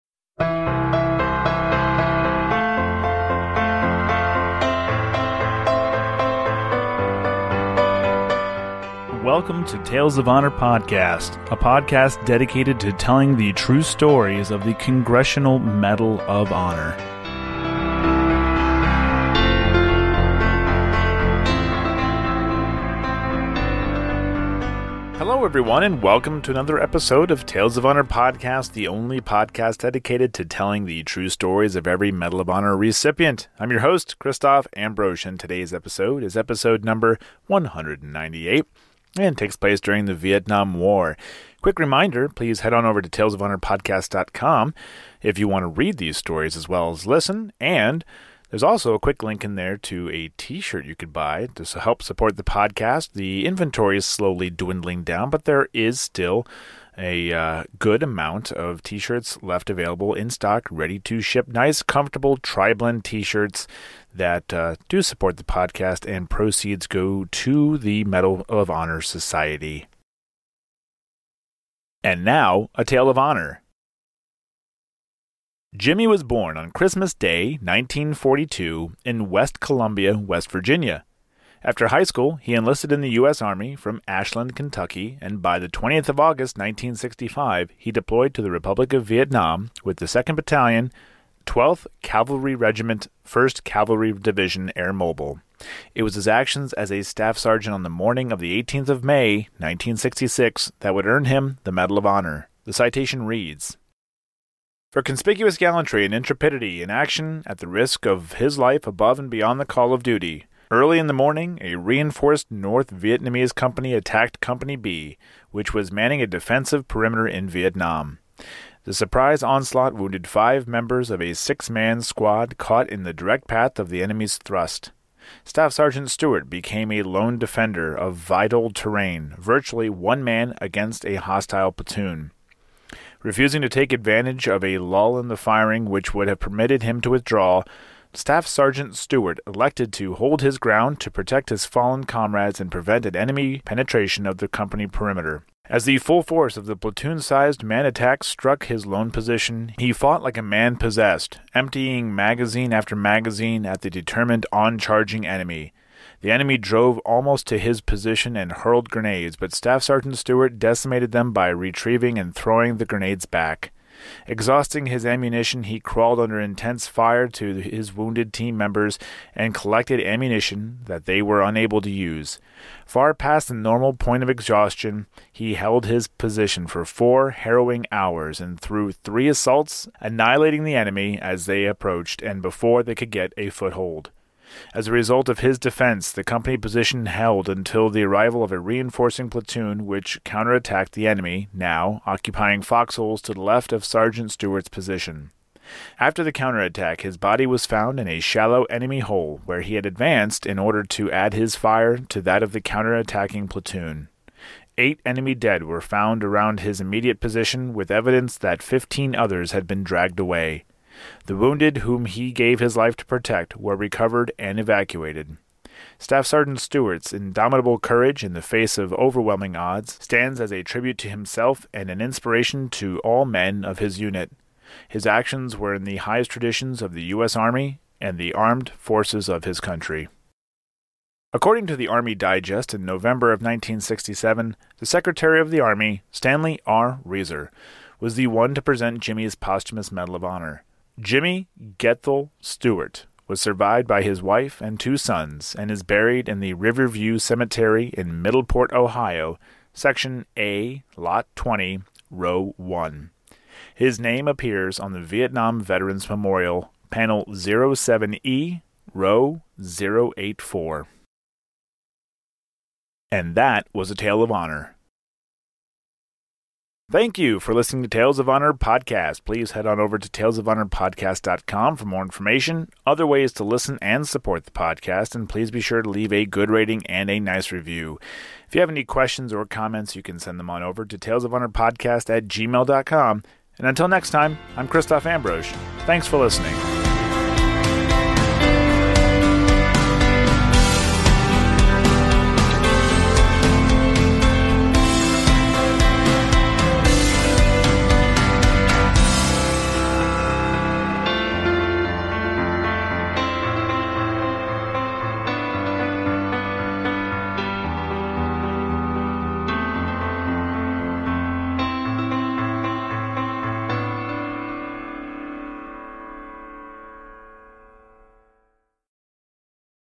Jimmy G Stewart - Medal of Honor Recipient
On episode one hundred and ninety-eight, the story of Jimmy G Stewart is told.

 All stories in April and May will be from the Vietnam War.